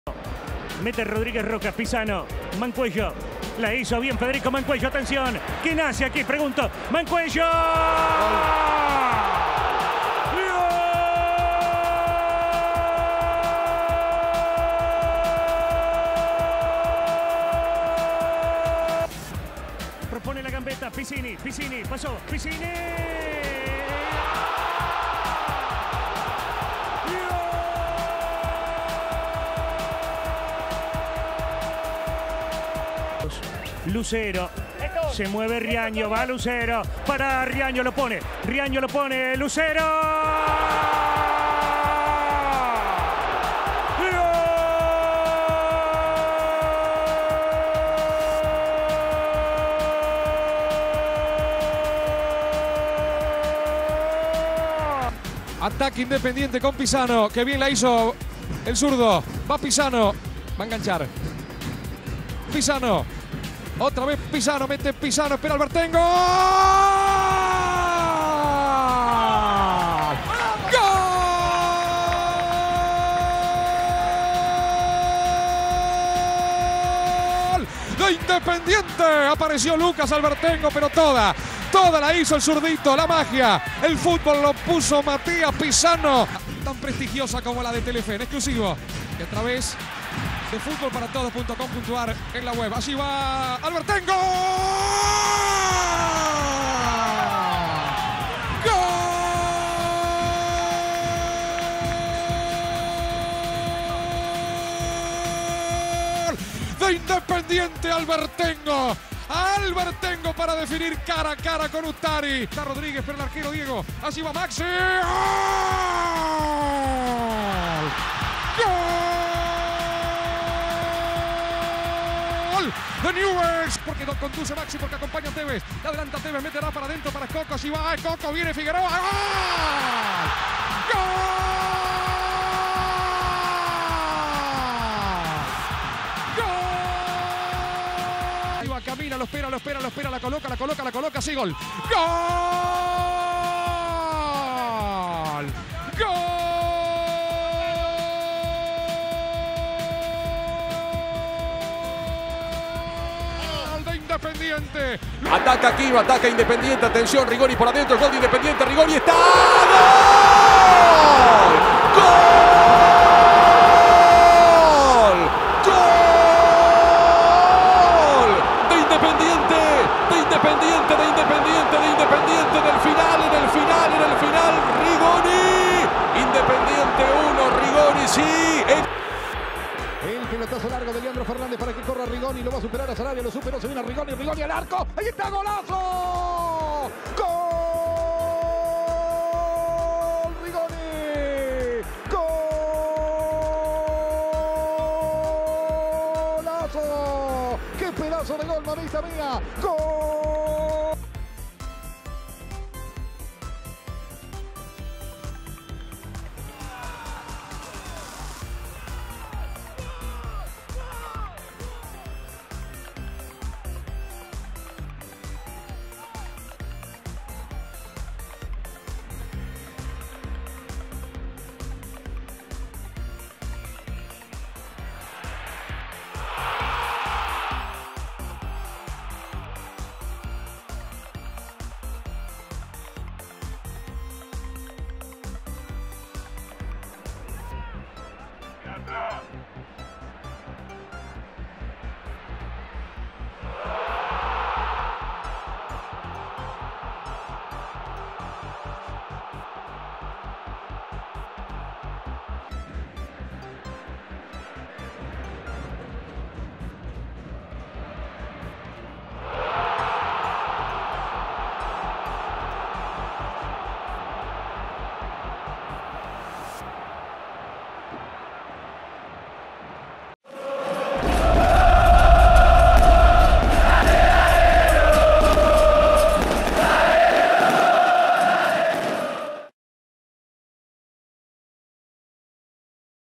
Los5 últimos debuts en torneos locales de Independiente
1) Transición 2014: Independiente 3-0 Atlético Rafaela (F. Mancuello, F. Pizzini y J.M. Lucero)
2) Campeonato 2015: Newell's 2-3 Independiente (M. Rodríguez y V. Figueroa; L. Albertengo -2- y D. Rodríguez)
3) Transición 2016: Independiente 1-0 Belgrano (E. Rigoni)
4) Torneo 2016/2017: Belgrano 0-1 Independiente (E. Rigoni)
5) Superliga 2017/2018: Independiente 3-1 Huracán (M. Benítez -2- y L. Fernández; R. Ábila)